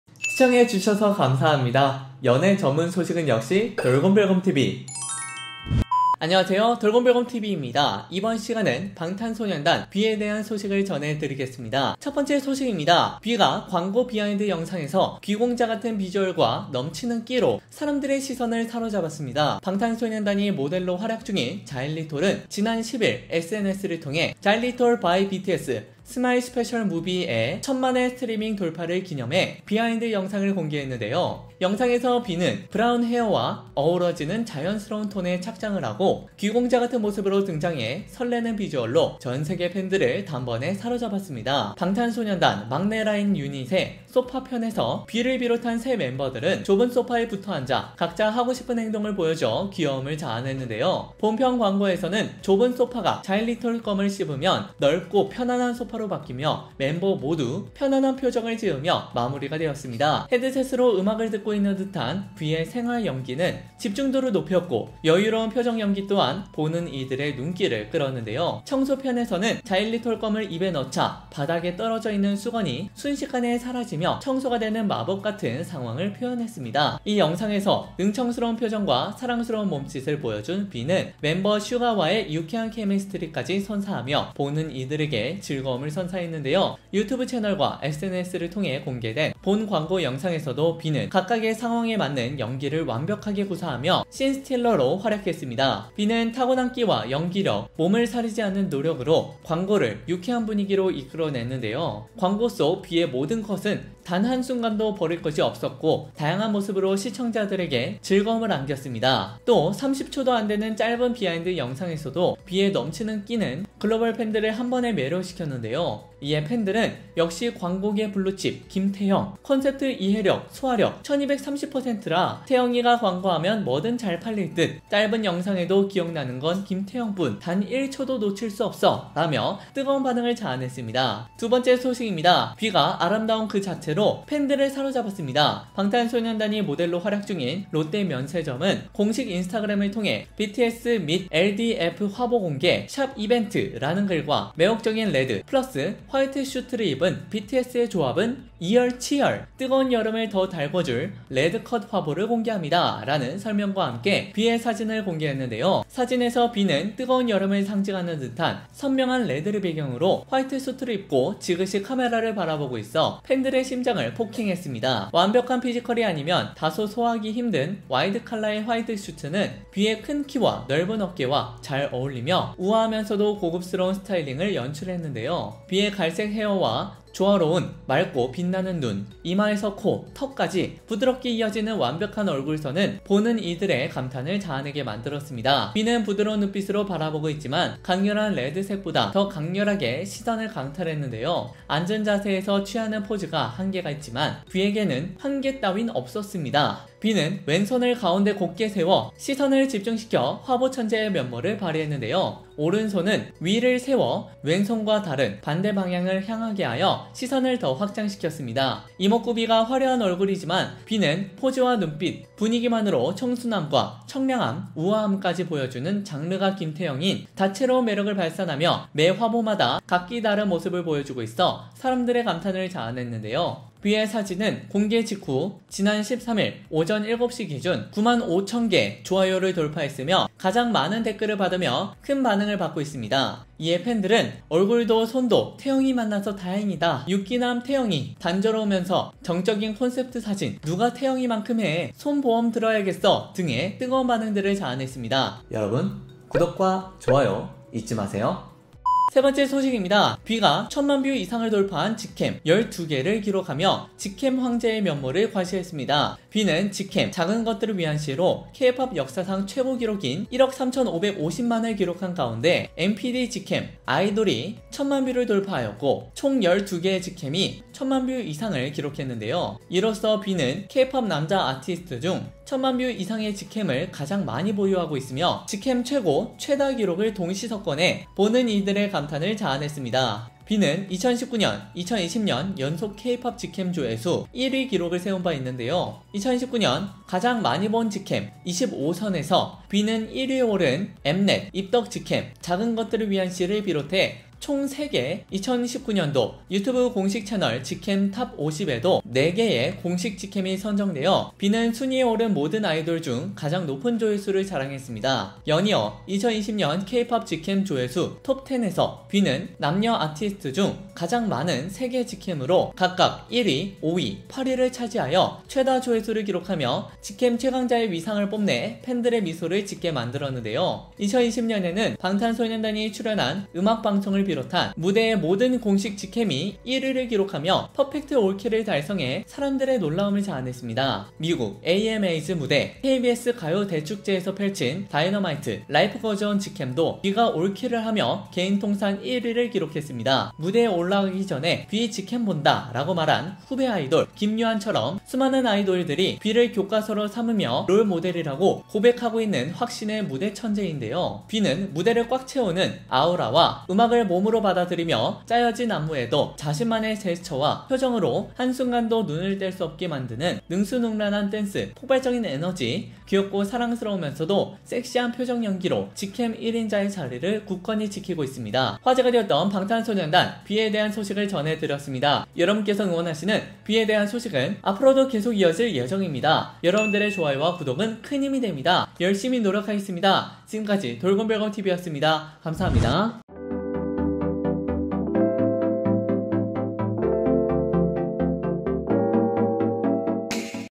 【ENG】BTS 뷔, 자일리톨과 방탄소년단의 콜라보 광고 영상 천만뷰 돌파 기념!! 비하인드 영상 공개!! 귀공자 비주얼에 생활연기 미쳤다 BTS V 돌곰별곰TV
감사합니다.

네이버 파파고 다국어 번역 :

BTS V celebrates 10 million views on Xylitol and BTS's collaboration commercial! The behind-the-scenes video! You look like a prince and you're acting like a prince.

BTSヴィ、キシリトールとBTSのコラボ広告映像千万ビュー突破記念！！ ビハインド映像公開！！ 貴公子のビジュアルに生活演技は狂っている。

紀念BTS V, 木糖醇和防彈少年團的合作廣告視頻突破千萬點擊量!! 公開幕後花絮視頻！ 貴公子的顏值讓生活演技瘋狂